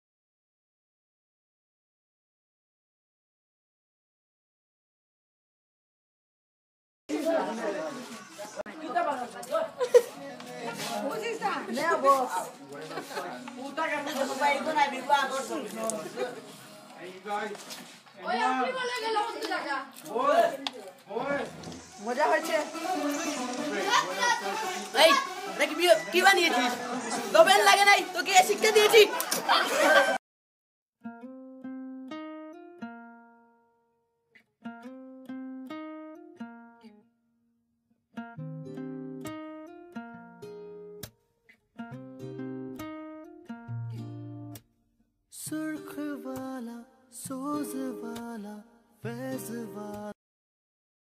Mistakes in making pizza funny 2015
chef getting slap by costumer